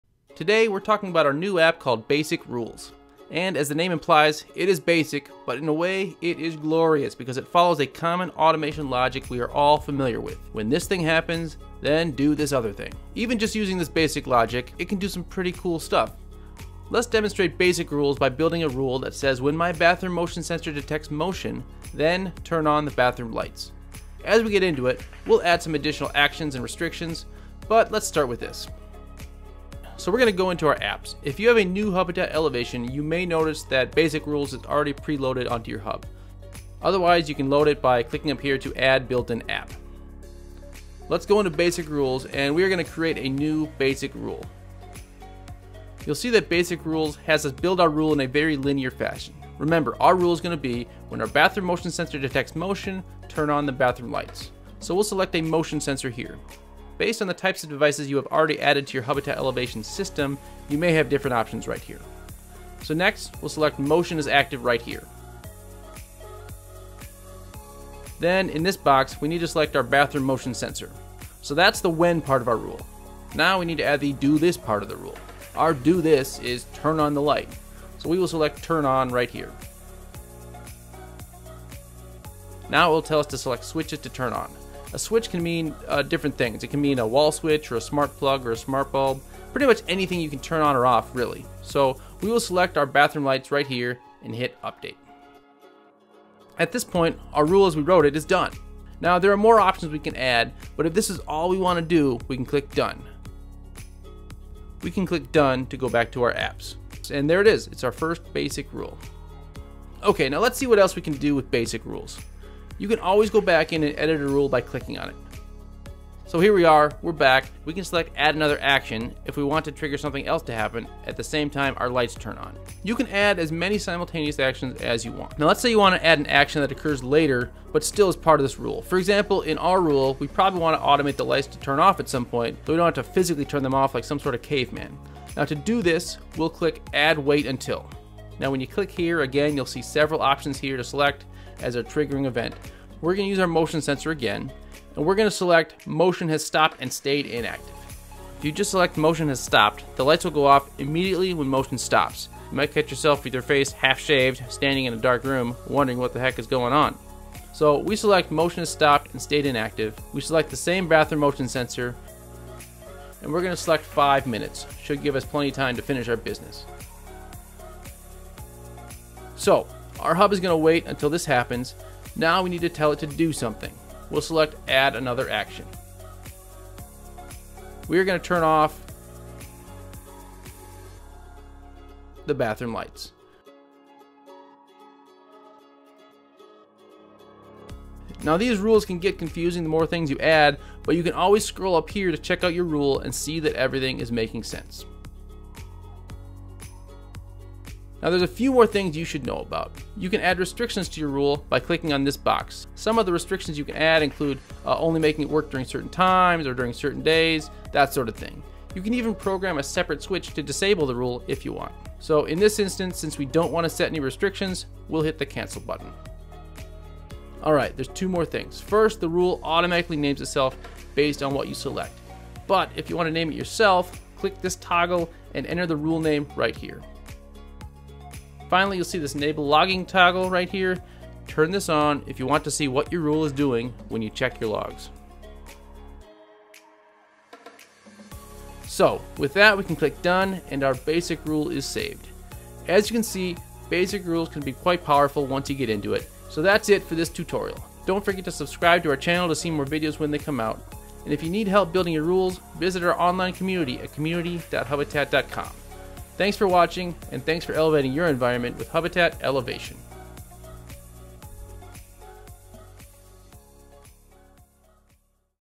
How to Use Basic Rules in Hubitat Elevation Tutorial
Hubitat Elevation Basic Rules is a new pre-installed app for automating your home. It follows a very familiar home automation logic: When X happens, then do Y. Basic Rules is a great app for those just getting started in home automation or for those looking for a quick way to build rules for their smart home.